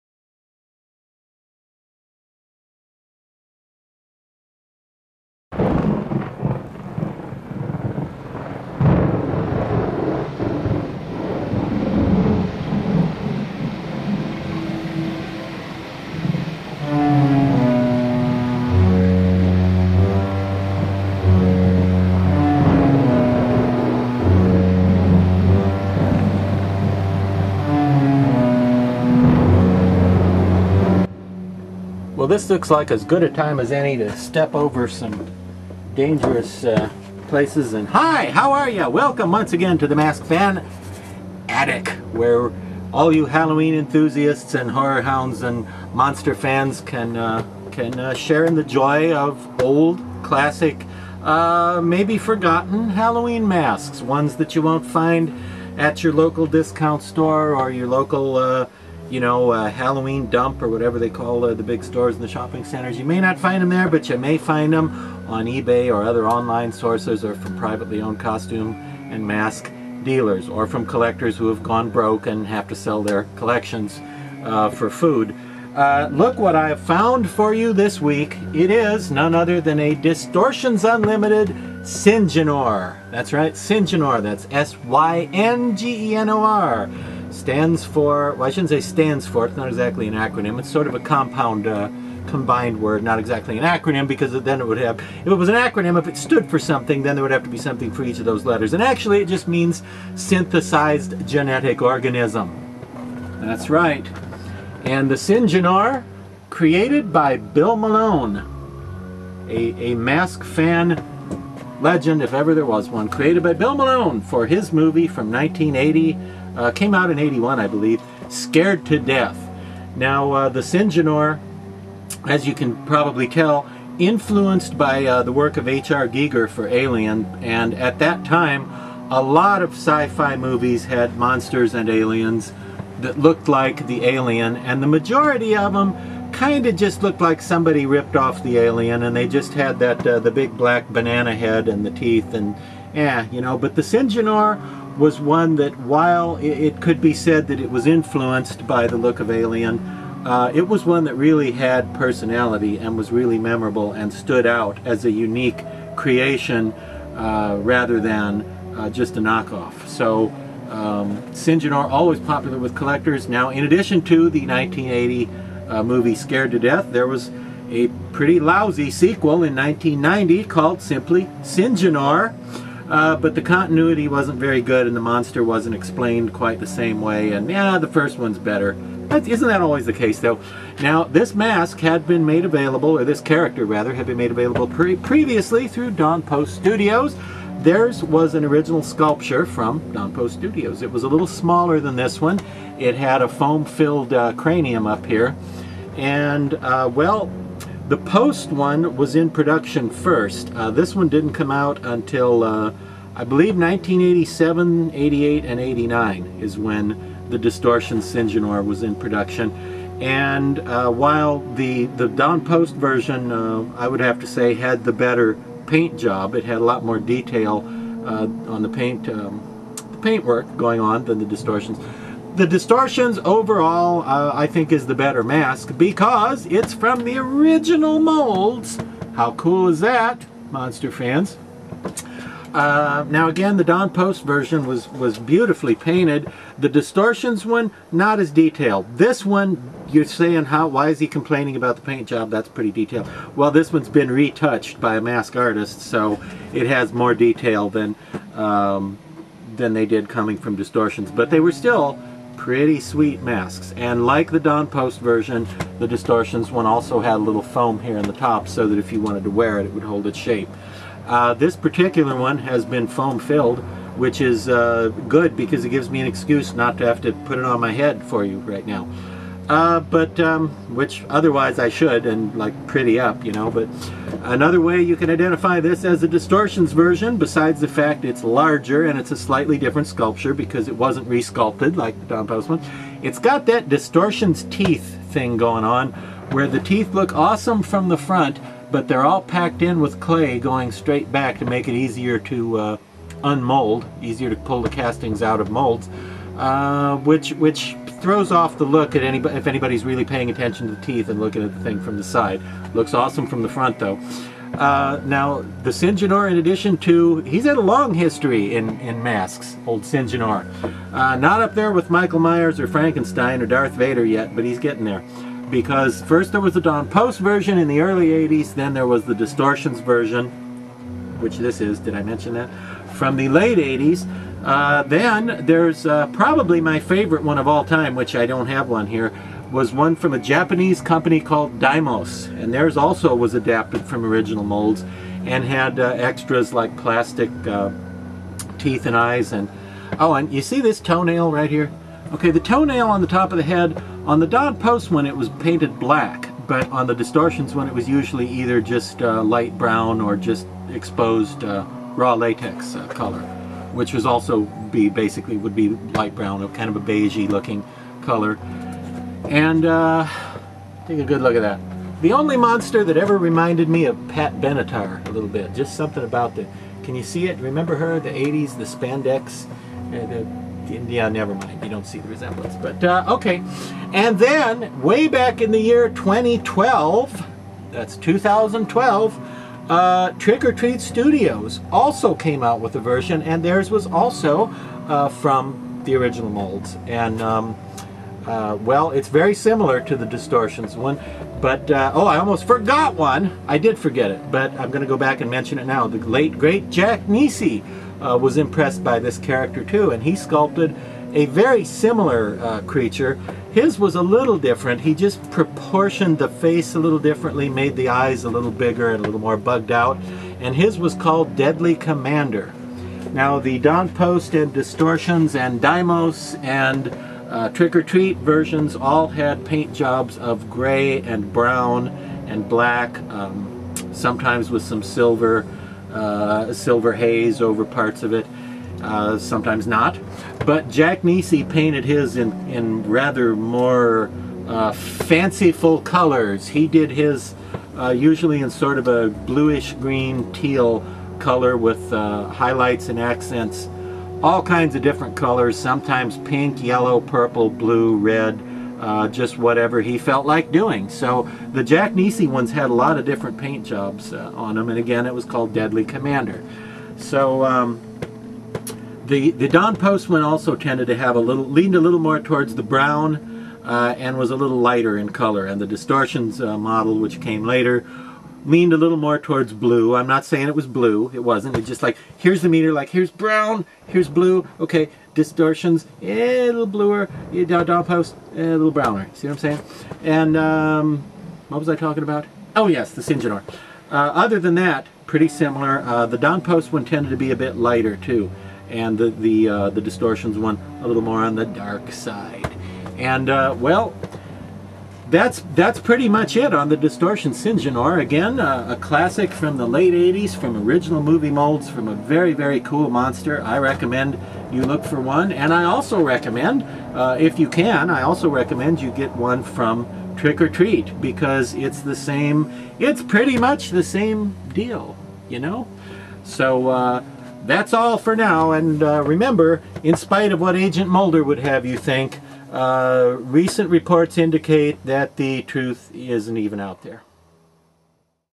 Mask Fan Attic: Syngenor by Distortions
DR LADY creeps up to the attic to find a rare Distortions Unlimited mask of Bill Malone's SYNGENOR.